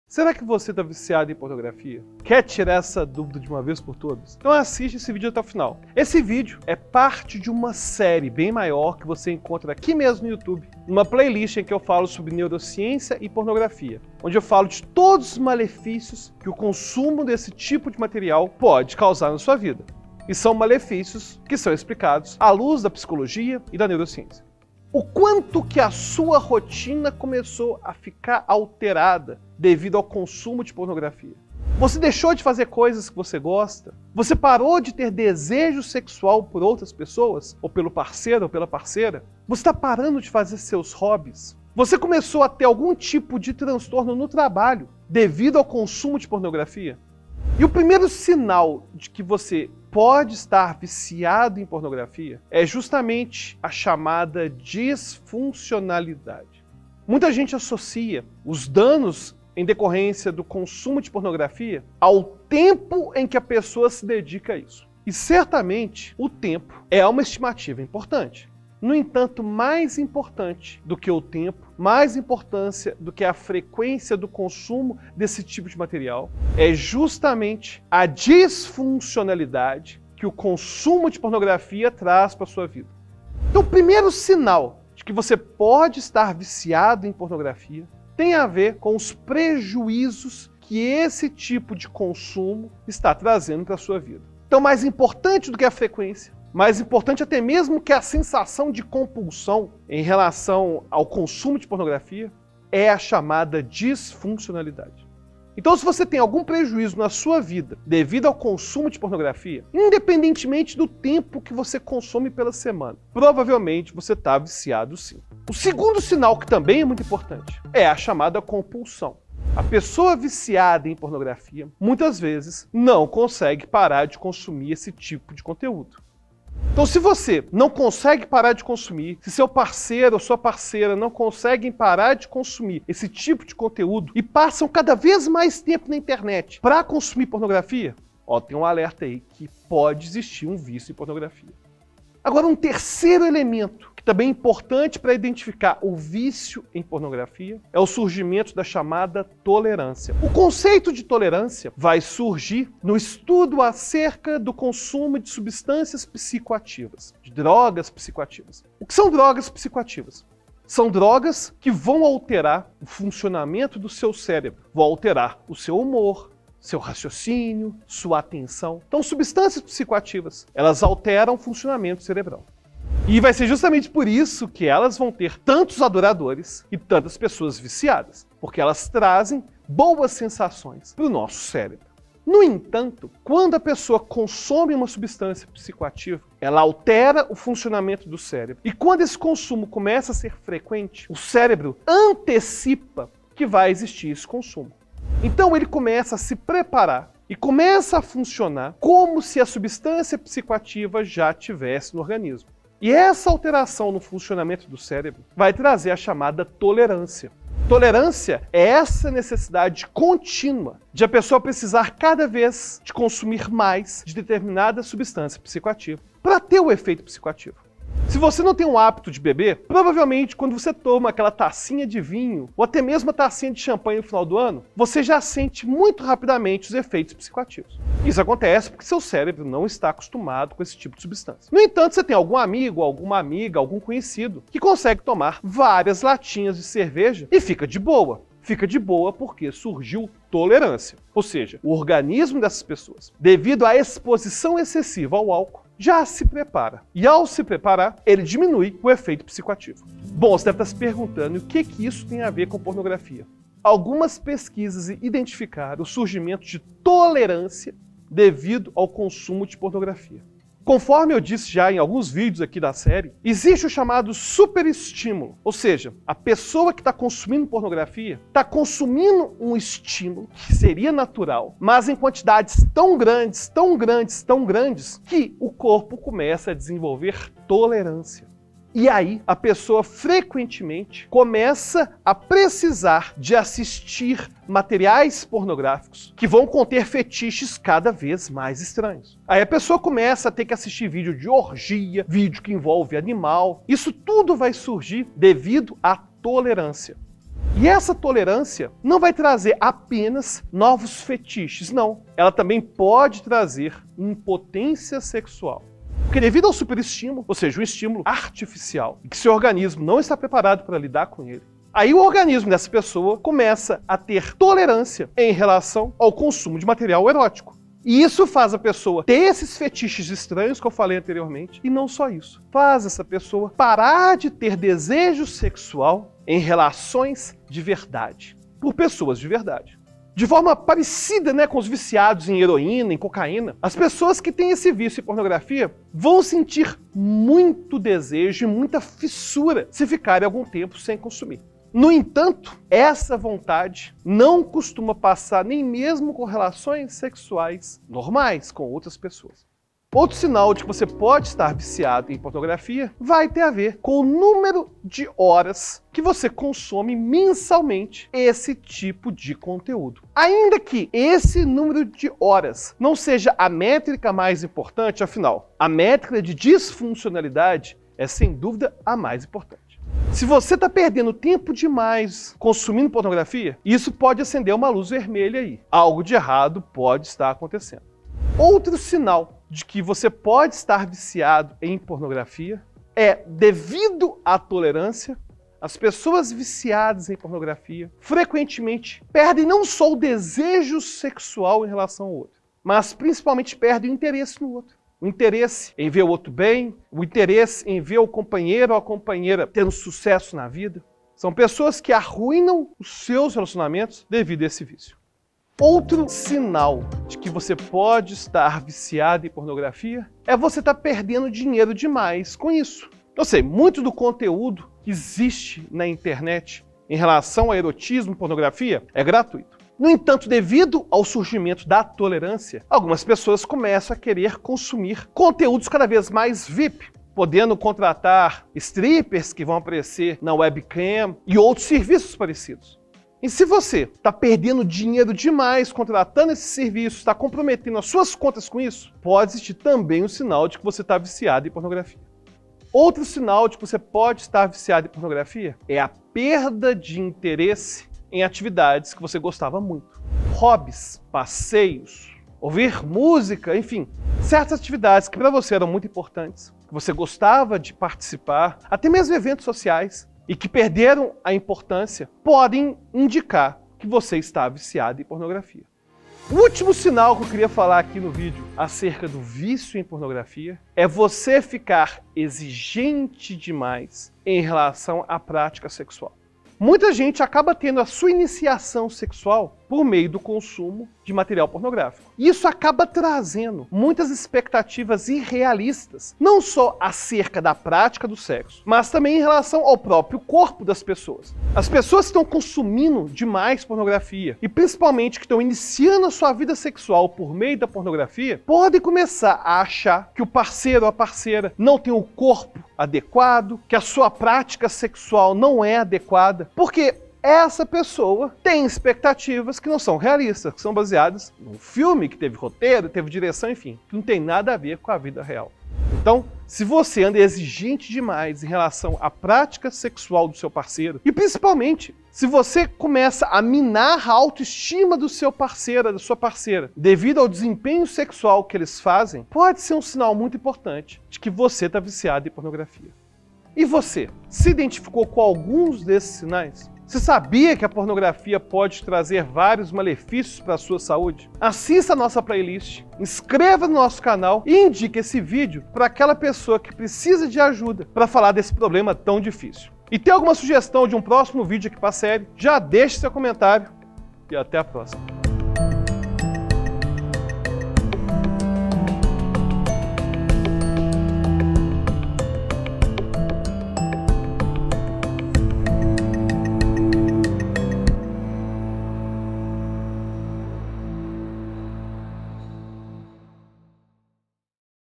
SINAIS DO VÍCIO EM P0RNOGRAFIA
Como saber que você está viciado em pornografia? Deixou de fazer determinadas atividades por causa dela? A disfuncionalidade mexe com seu cérebro e mostra o quanto você já está completamente viciado nisso.

Saiba no vídeo de hoje como passar por isso e quais ferramentas pode usar para se precaver desse vício que pode destruir seu cotidiano.